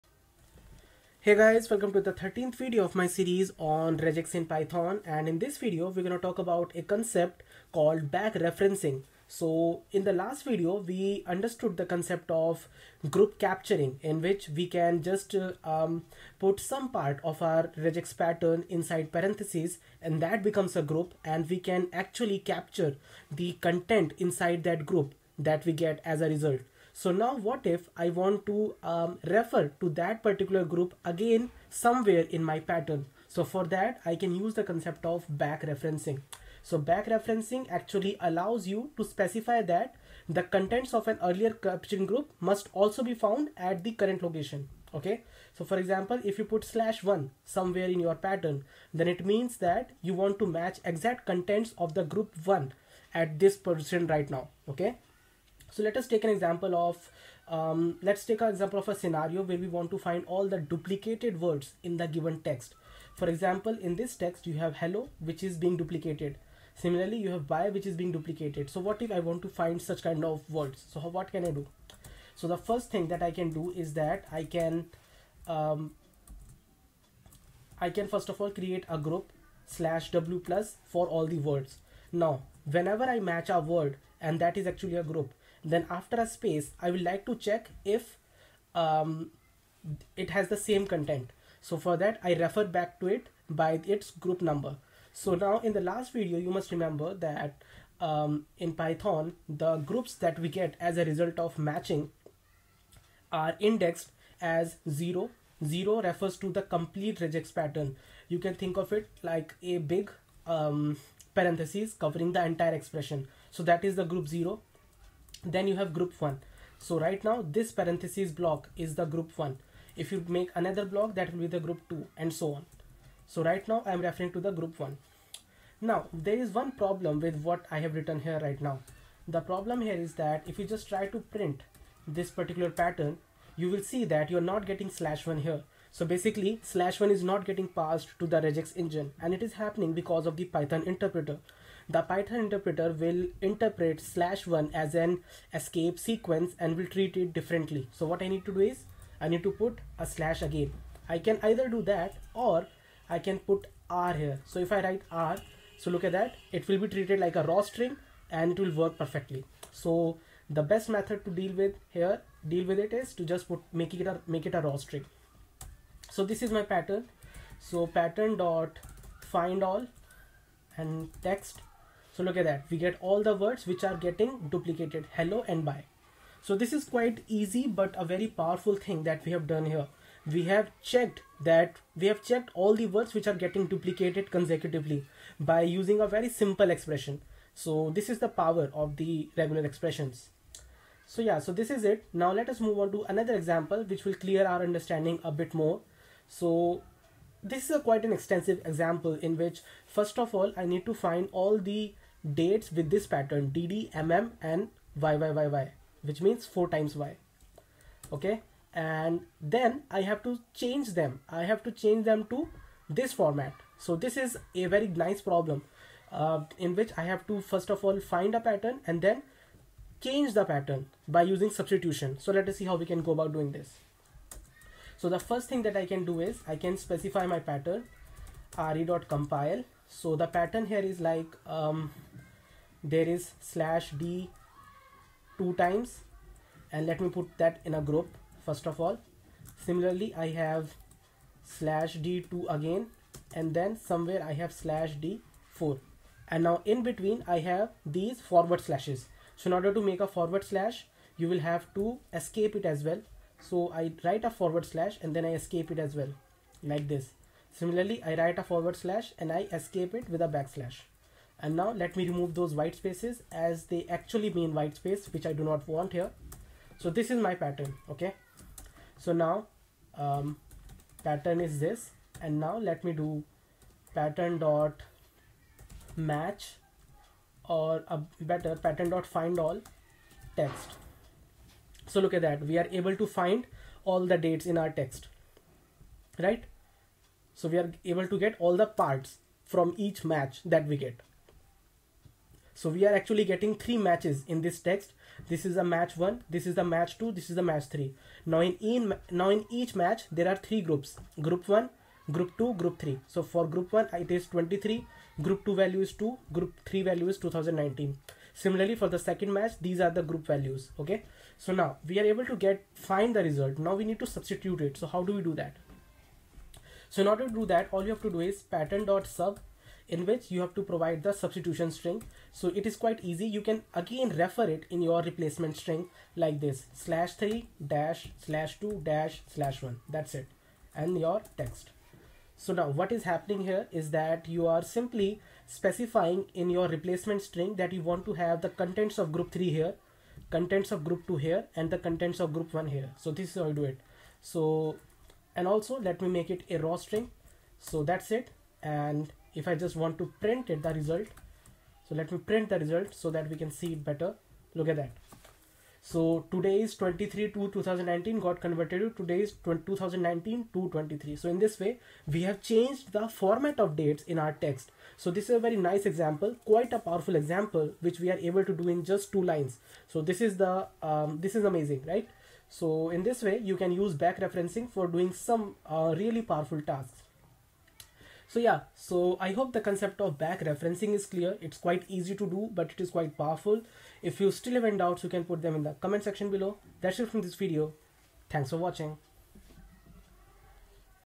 RegEx in Python (Part-13) | Backreferencing
Learn how to backreference groups in this video.

Backreferences in a pattern allow you to specify that the contents of an earlier capturing group must also be found at the current location in the string.

For example, \1 will succeed if the exact contents of group 1 can be found at the current position, and fails otherwise.